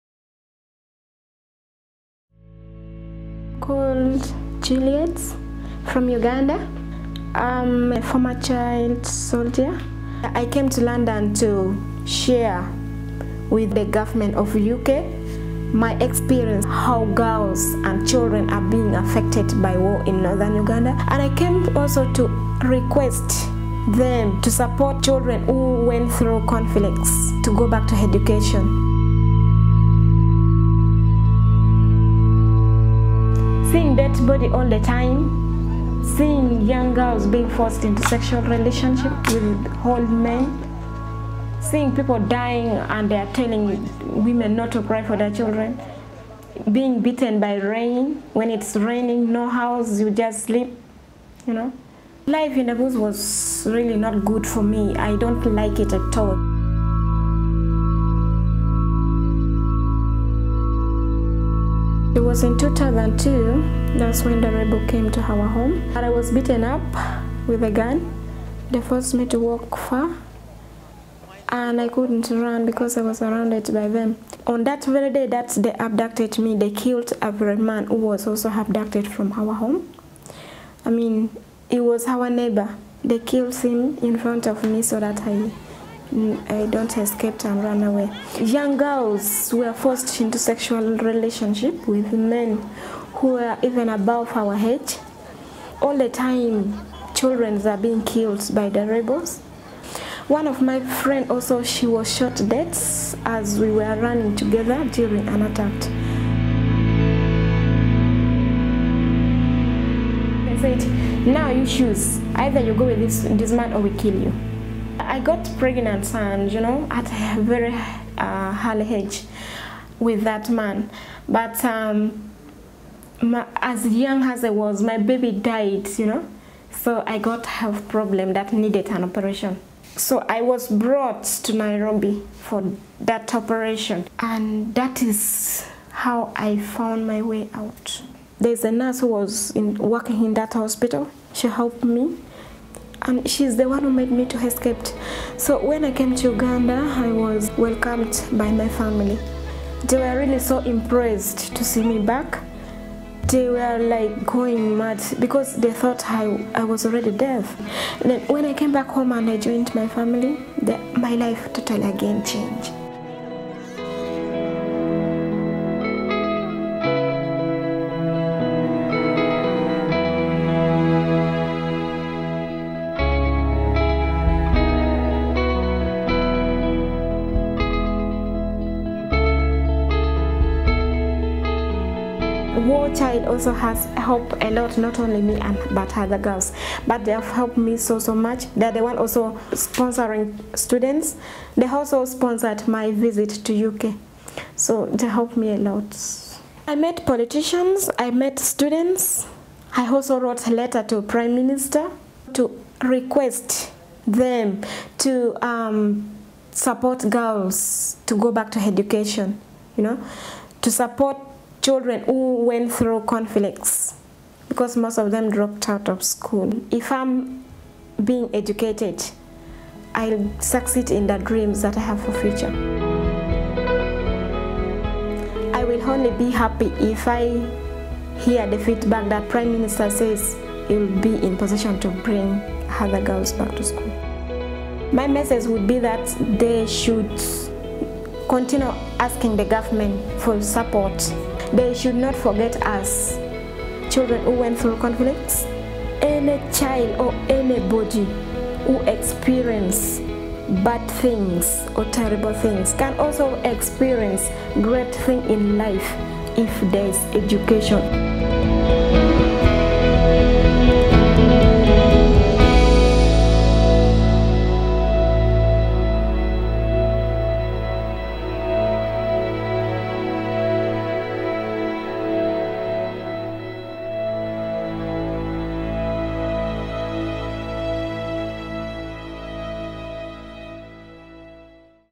Juliet's journey from child soldier to global campaigner
Juliet was abducted by Joseph Kony's Lord's Resistance Army and forced to become a 'wife' at the age of 12.

Unlike many children in similar situations, she managed to escape and return home to her family. With our support, she managed to return to school and made incredible progress.

In the hope of preventing other girls from having the same terrible experience, she visited London with us to help campaign for other girls to be given the chance to access an education.

During her trip she delivered a letter to 10 Downing Street, met with politicians and civil servants, appeared on CNN, Teachers TV and 4 BBC radio interviews, and also visited some schools to inspire students with her incredible story.